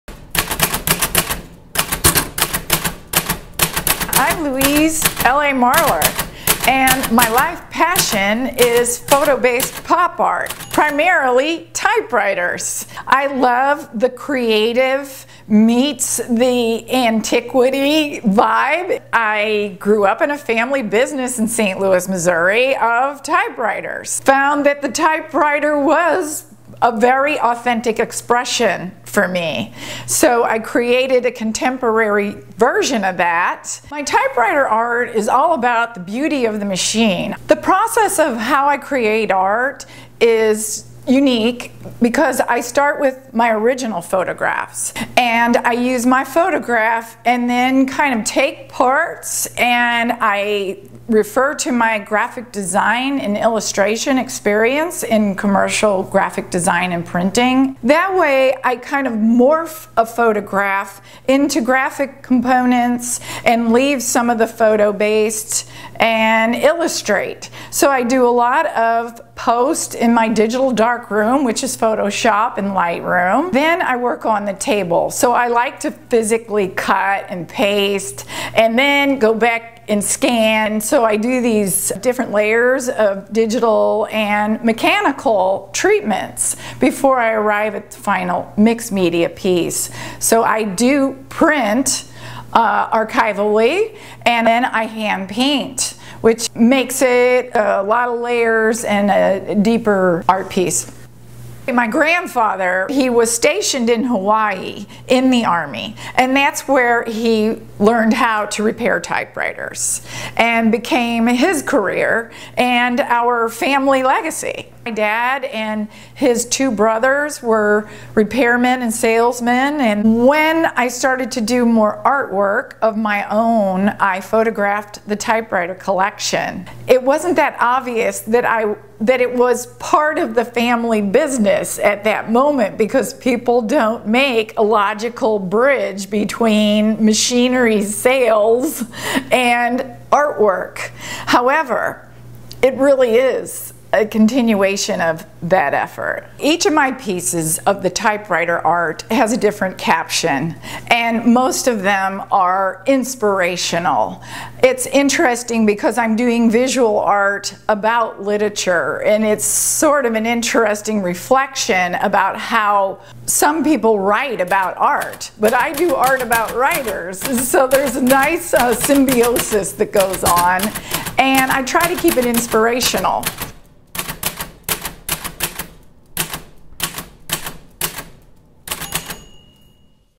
California Typewriter Artist Story - history and process
LA Marler is a high tech digital artist whose themes include analog media. Her artwork has a consistent dialog between the retro media tools and our ever-changing digital lives of self publishing on the internet.